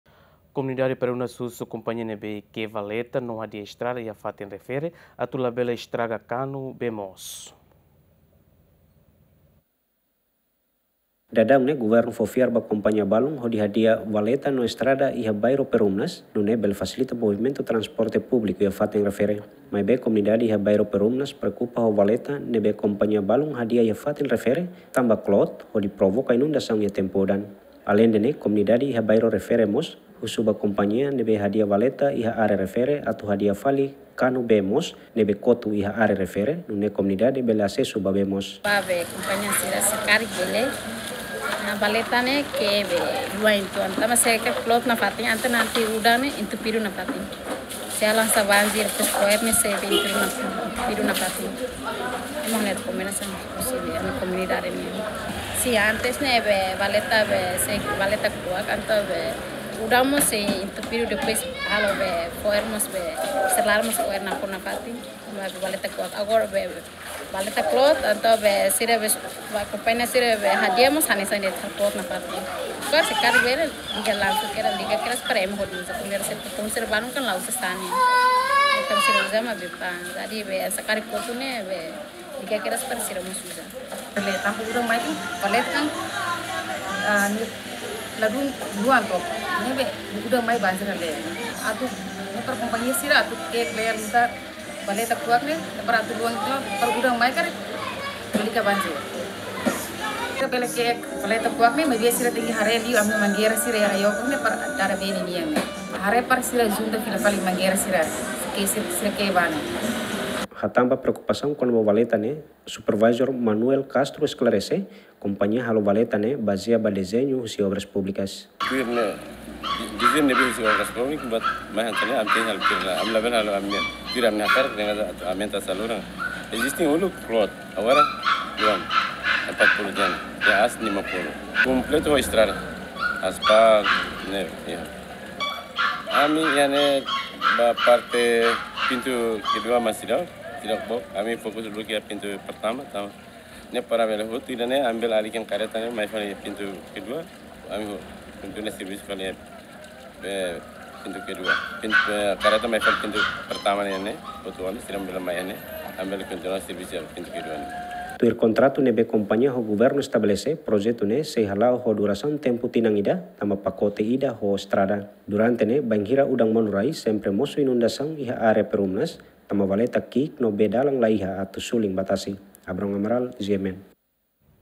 Komunidade Perumnas husu kompania ne’ebé kee valeta no hadia Estrada atu labele estraga kanu...
Komunidade Perumnas husu  kompania ne’ebé kee valeta no hadia Estrada iha fatin refere atu labele estraga kanu bee mos